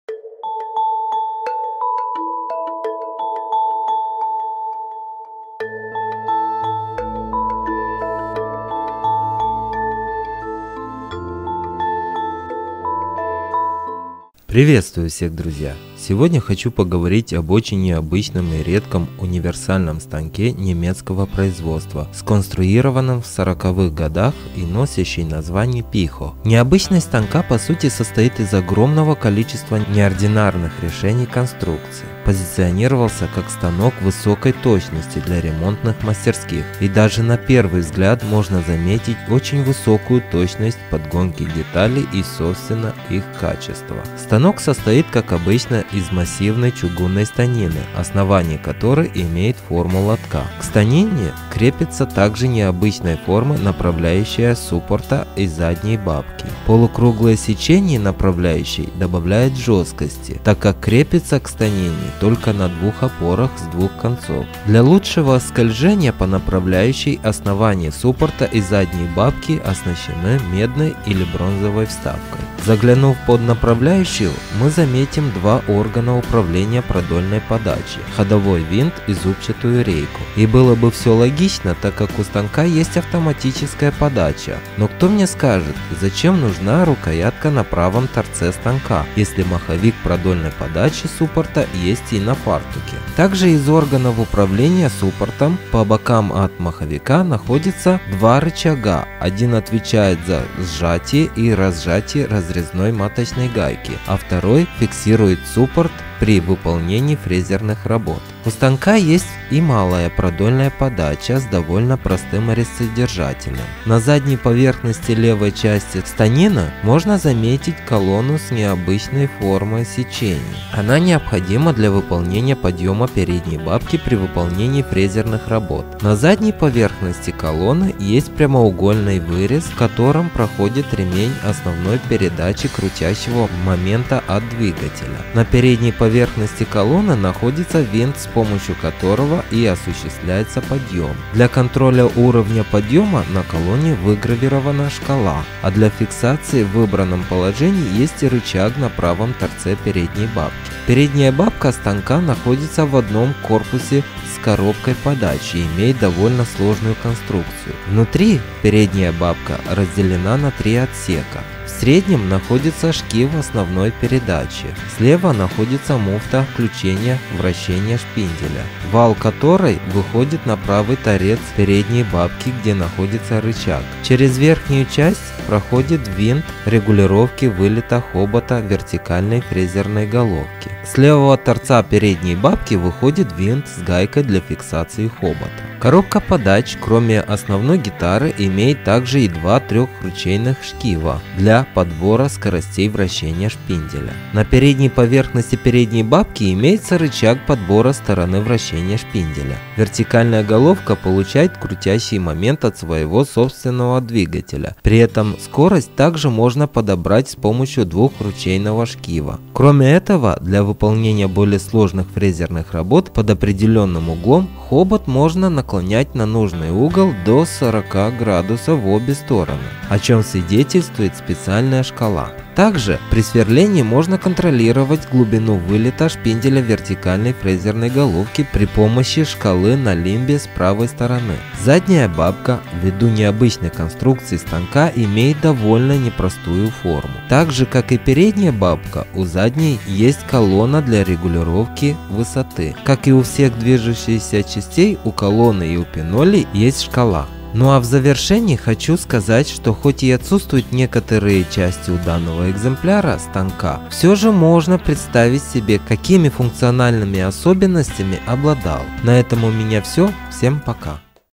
Очень необычный немецкий станок /|\ Very unusual German machine tool
Универсальный металлообрабатывающий станок 40-х годов производства германии - Piho.